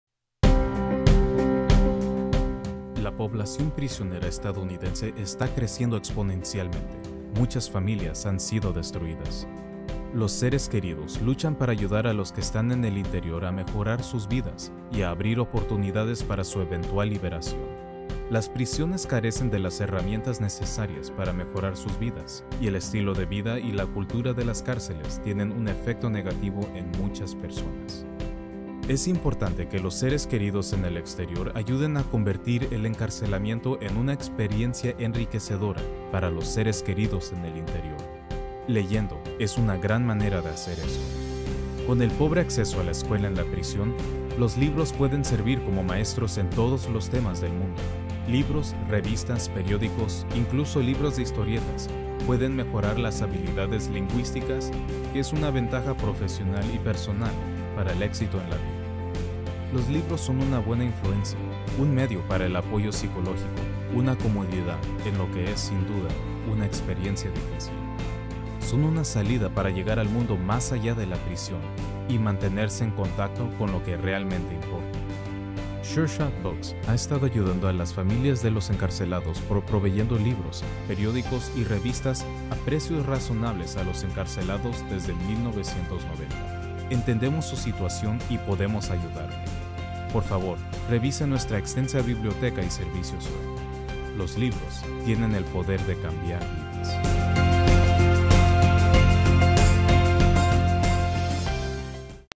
Sure Shot Books v6 Spanish
¿Puedes proporcionar la versión en español en cada versión en inglés?
1. SureShotBooks Publishing LLC Somos una librería en línea que sirve a la población reclusa con los Mejores Libros, Revistas & Periódicos. Prestamos servicio a Todas las Instalaciones Correccionales a través de
2. En SureShot Books Publishing LLC nos esforzamos por brindarles a los reclusos hispanos la oportunidad de adquirir conocimientos aprendiendo inglés, leyendo libros o cualquier cosa de interés que impulse la alfabetización básica.
3. SureShot Books Publishing LLC fue fundada en 1990 para ayudar a las familias de los presos hispanos, al ofrecer Libros, Periódicos & Revistas a precios razonables.
4. En SureShot Books Publishing LLC nos esforzamos por brindarles a todos los reclusos la oportunidad de adquirir conocimientos, al proporcionarles material de lectura actual que les ayude a concienciar sobre la vida cotidiana. Para que la educación esté disponible para todos los reclusos, brindamos una amplia selección de Libros Bíblicos, Libros de Derecho, Salud & Ejercicio, de Autoayuda, Revistas, Periódicos Y Mucho Más. Visítenos en